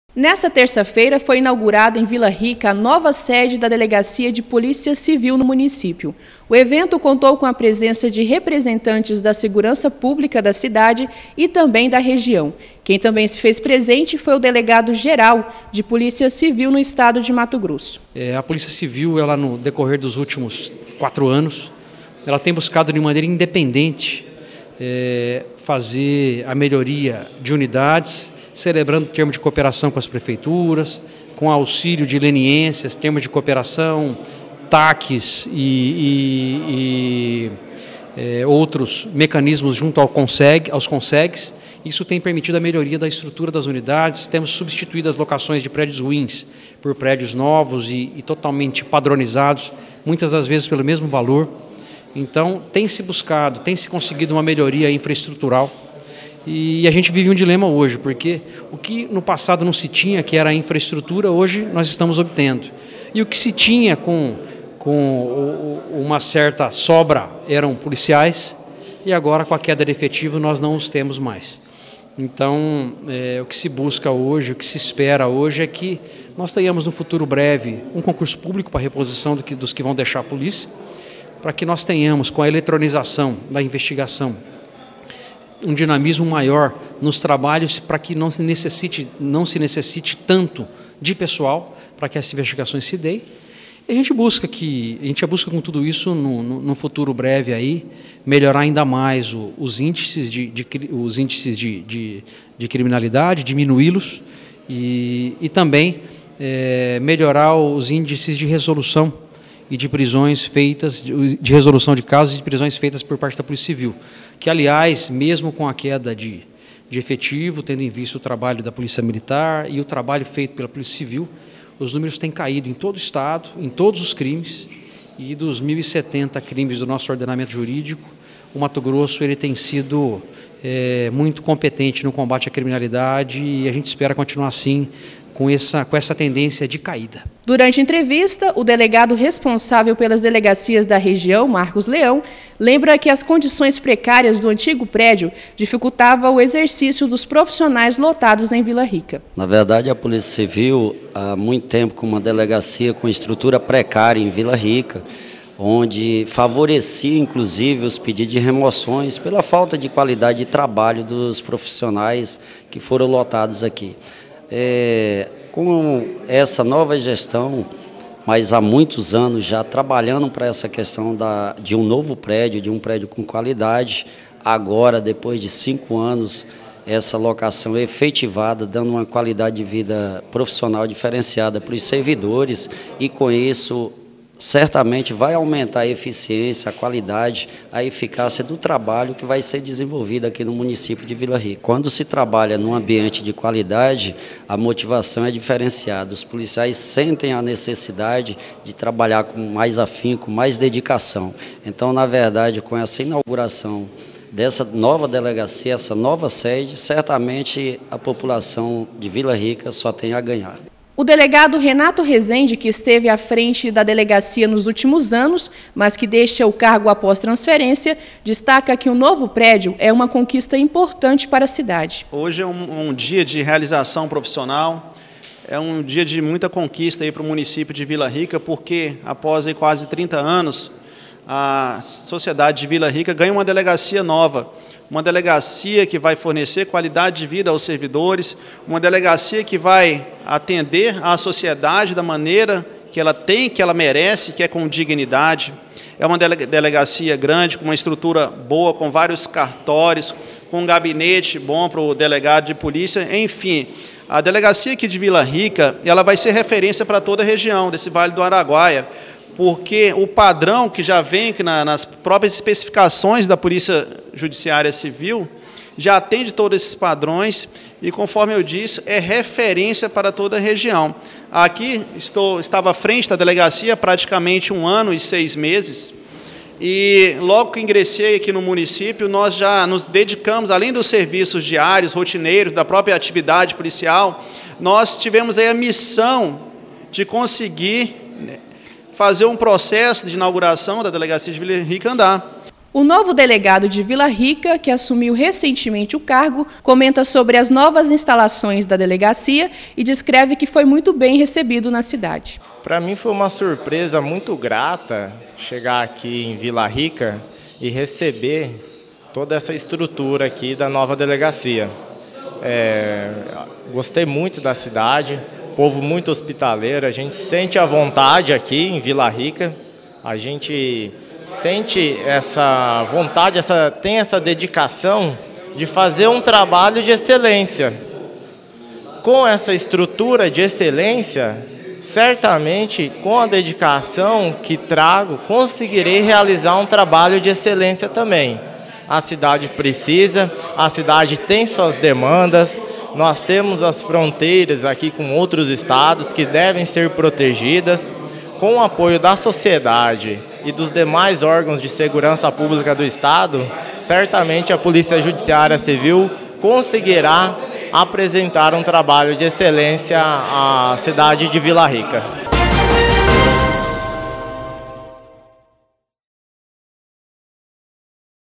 Novo prédio da delegacia de Polícia Civil é inaugurado em Vila Rica
A delegacia de Polícia Civil em Vila Rica foi conhecida por muitos anos por sua precariedade, o que acabava influenciando no trabalho realizado. Com a inauguração do novo prédio ocorrida nesta terça-feira (09) acredita-se que esse cenário ficará no passado.